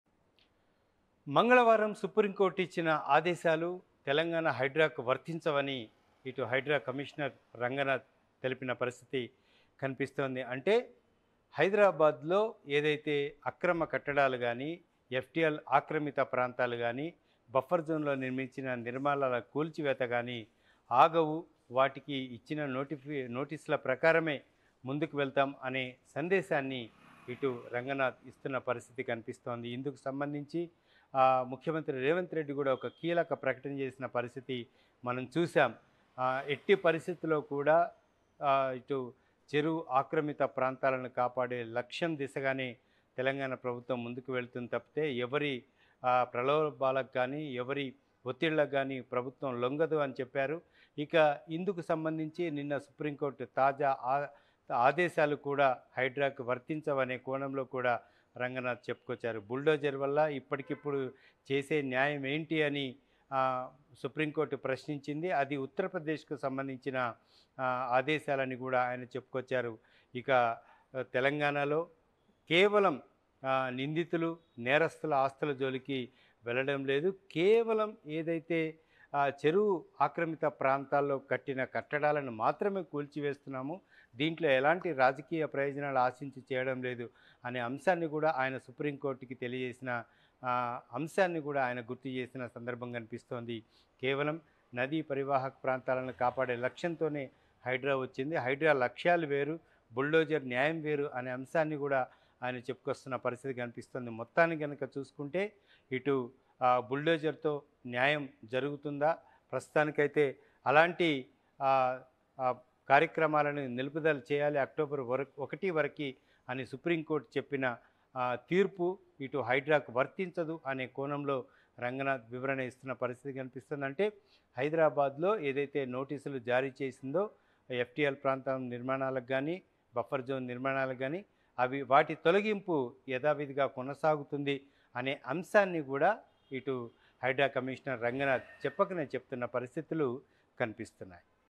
సుప్రీం బుల్డోజర్ న్యాయం ఆదేశాలు హైడ్రాకు కాదు.. వివరణ ఇచ్చిన కమీషన్ రంగనాధ్ | Oneindia Telugu
సుప్రీం కోర్టు బుల్డోజర్ న్యాయం ఆదేశాలు తెలంగాణ హైడ్రాకు వర్తించవని హైడ్రా కమీషనర్ రంగనాథ్ స్పస్టం చేసారు. అవినీతి పరులు, నేరస్తుల ఆస్తుల జోలికి హైడ్రా వెళ్లబోదని సుప్రీంకోర్ట్ కు వివరించినట్టు రంగనాథ్ తెలిపారు.
Hydra Commissioner Ranganath clarified that Supreme Court bulldozer justice orders are not applicable to Telangana Hydra. Ranganath said that he explained to the Supreme Court that HYDRA will not go after the properties of corrupt people and criminals.